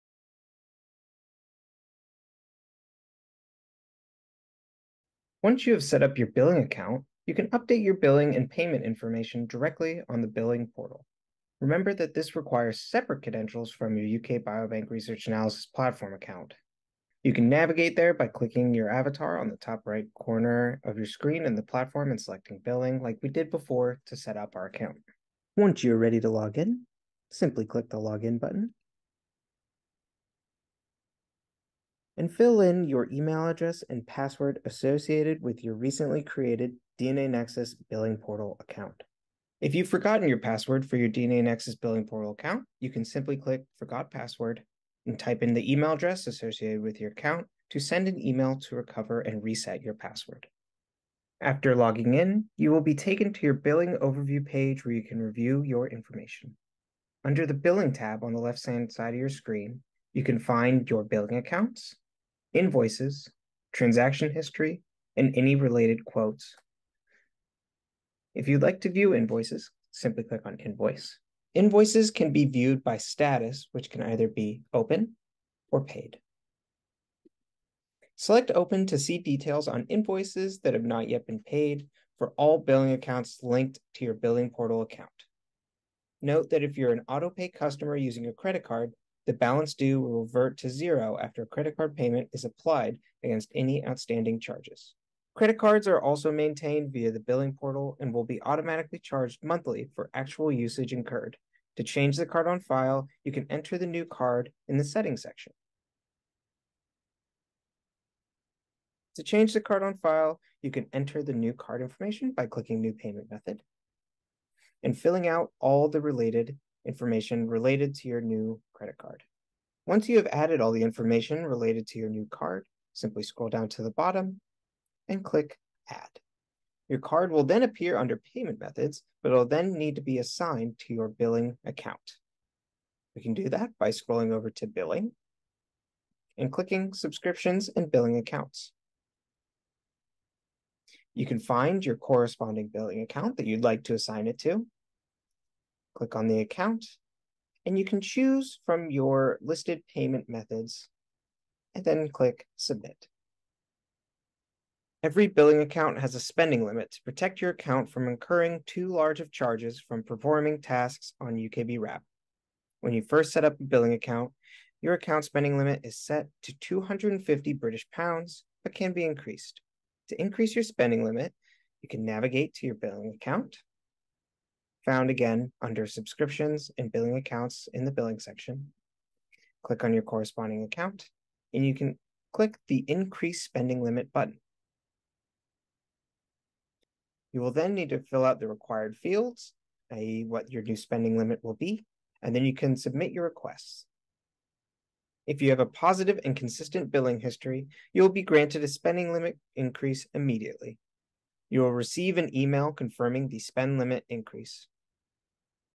Billing Portal Features: UK Biobank Research Analysis Platform
This tutorial takes you through the features available on the Billing Portal including how to update your payment information, view invoices and increasing your spending limit.